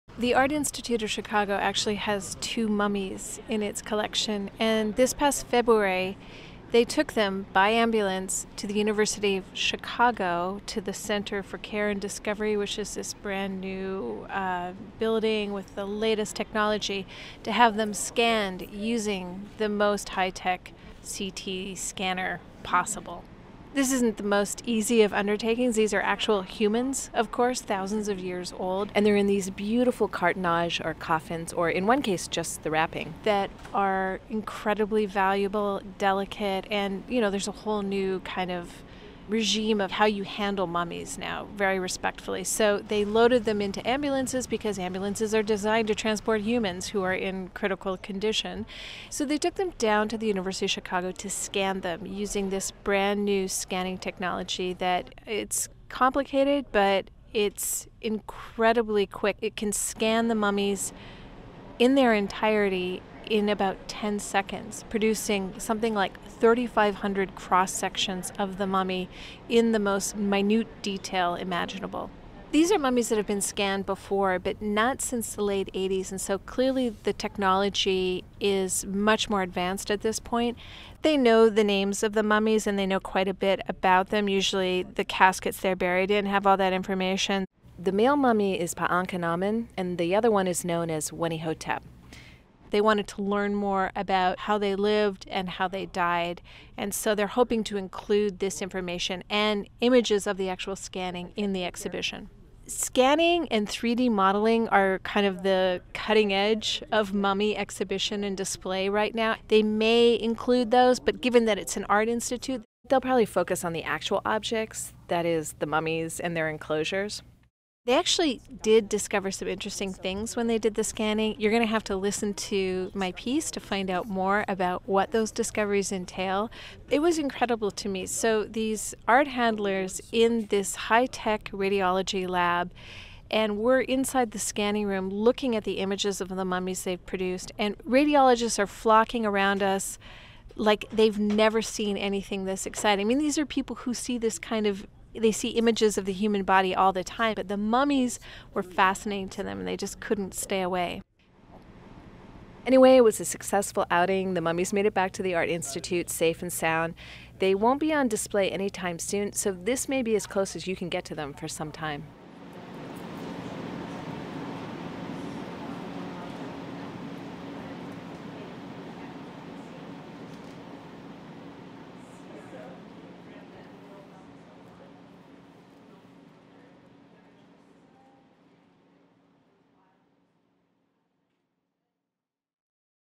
Secrets from the Tomb: Exploring Chicago's mummies
Chicago has three significant Egyptian collections, including a large number of mummies. Many have been in the city since the late 19th century. But thanks to new scanning technology, researchers are discovering new things about the long dead. WBEZ's Alison Cuddy explains.